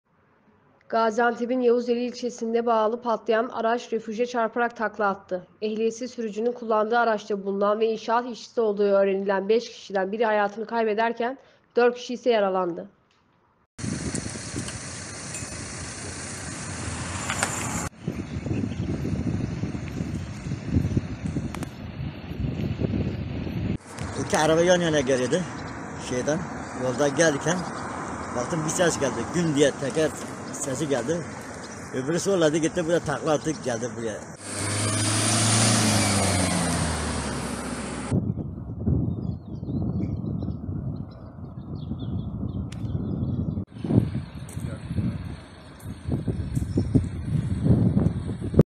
Gaziantep'te ehliyetsiz sürücü dehşeti
Lastiği patlayan araç takla attı: 1 ölü, 4 yaralı